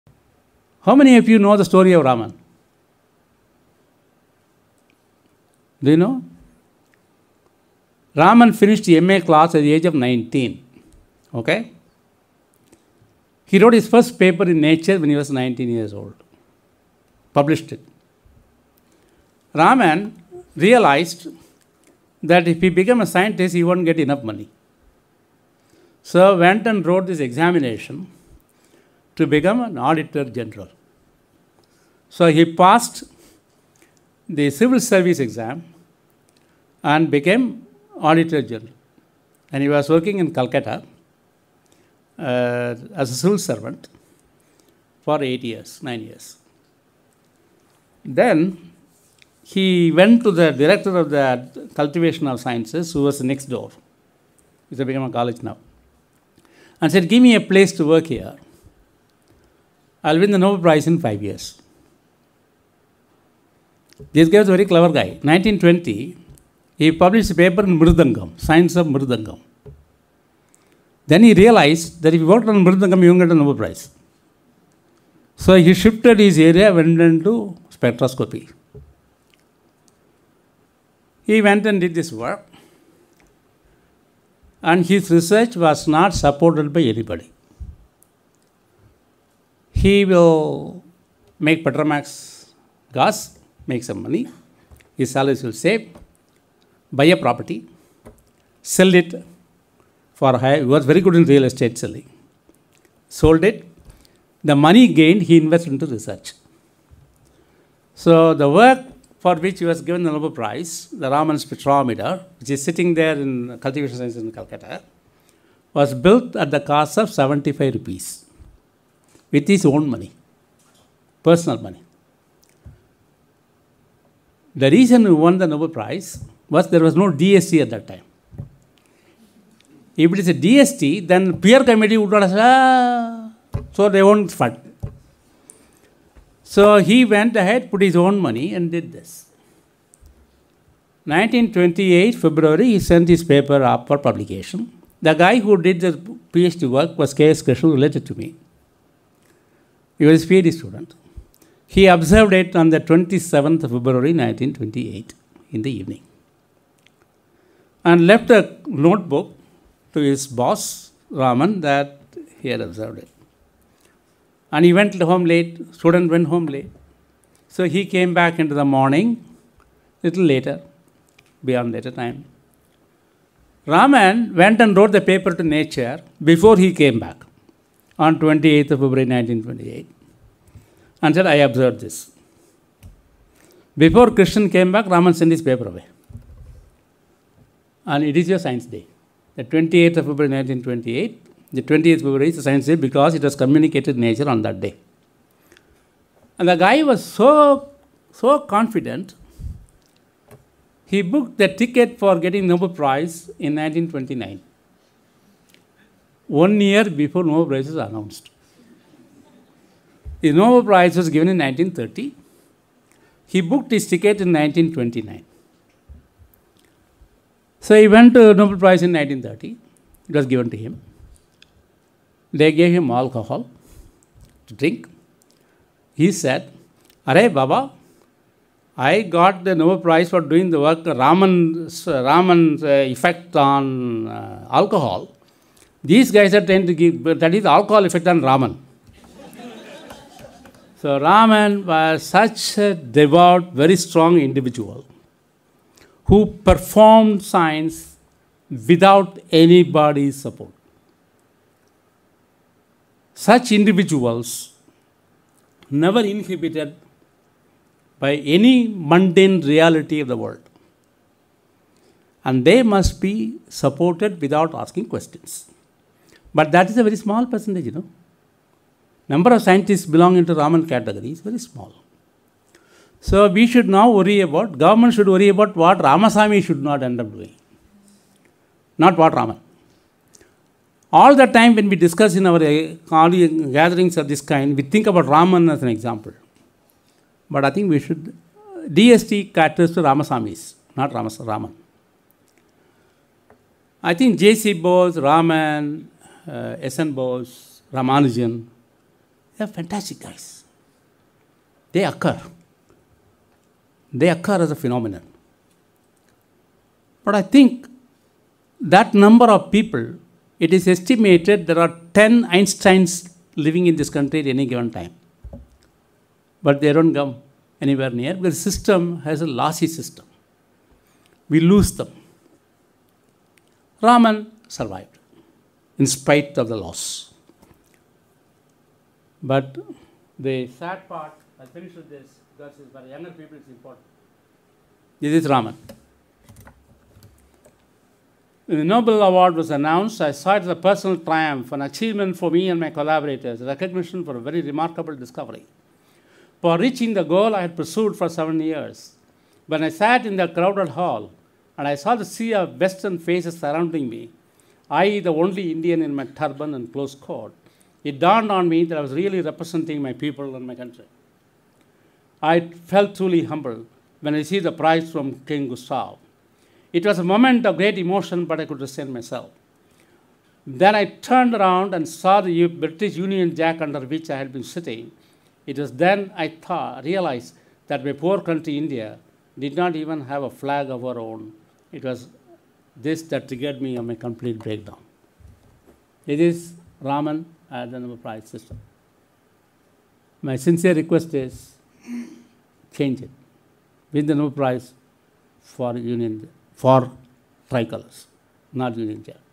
Story of C V Raman By :-T Ramasami
Dr. T Ramasami talk at IASST  at IASST on 3rd September 2017